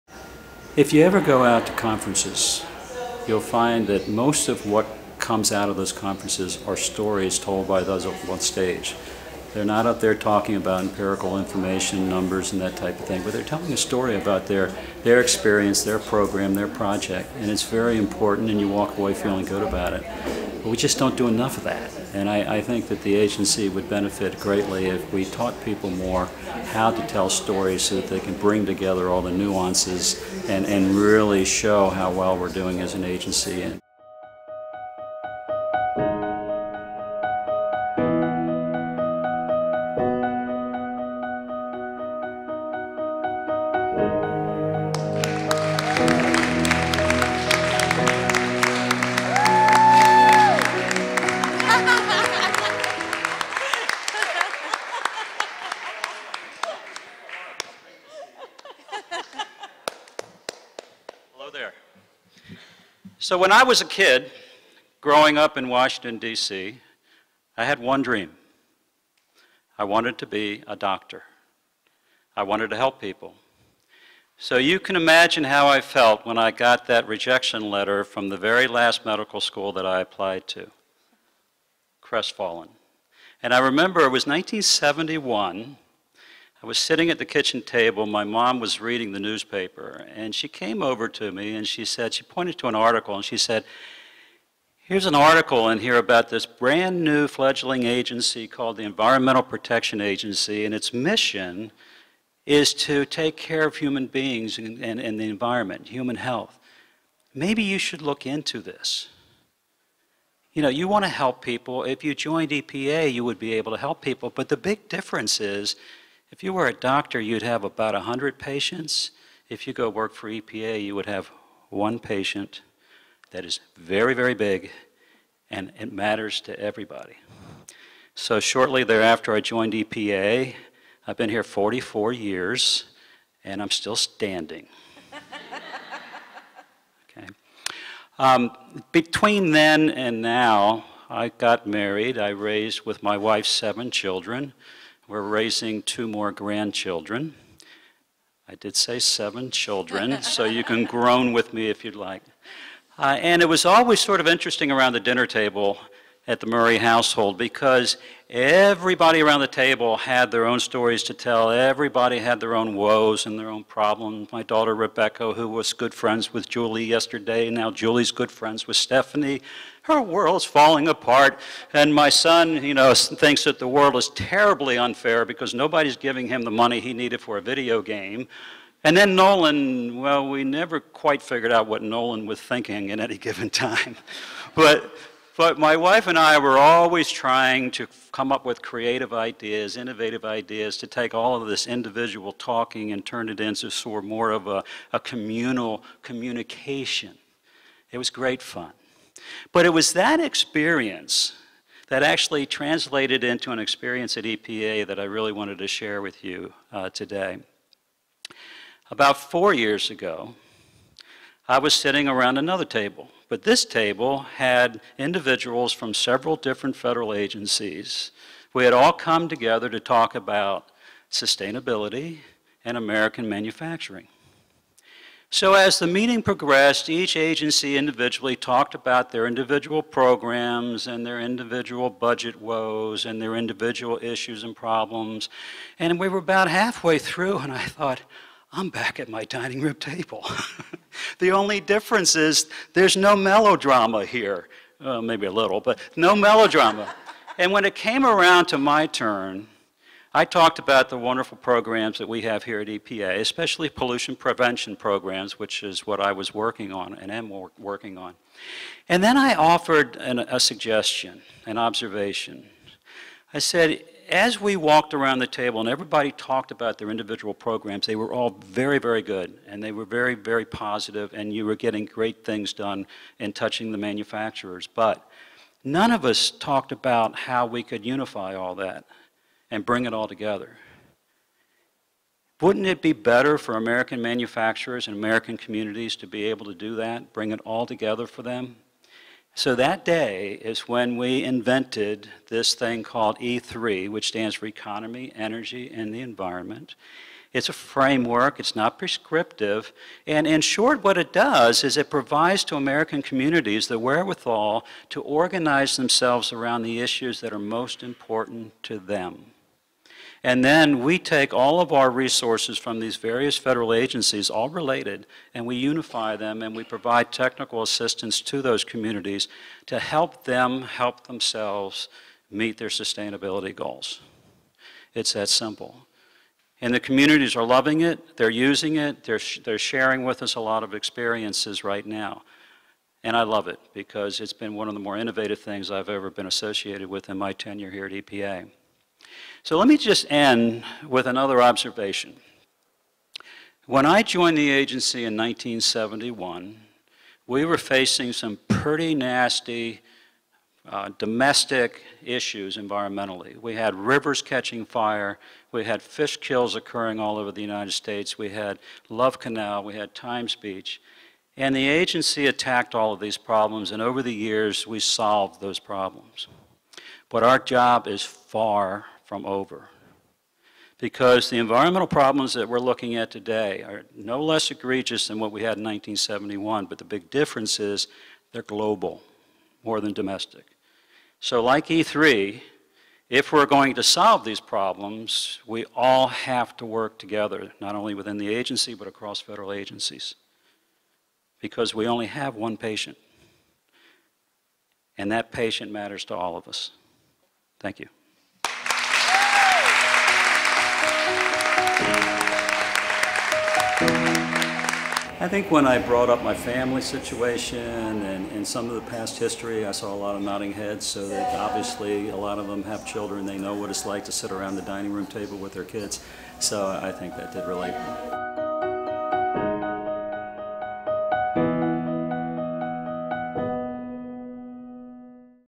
In One Take: E3 - The Economy, Energy and Environment Framework Tom Murray
The E3: Economy, Energy and Environment initiative is a federal technical assistance

framework helping communities, manufacturers and manufacturing supply chains adapt and thrive in today's green economy. EPA and five other federal agencies have pooled their resources to support small and medium-sized manufacturers with customized assessments. E3 is helping communities across the country reduce pollution and energy use while increasing profits and creating new job opportunities.

policy/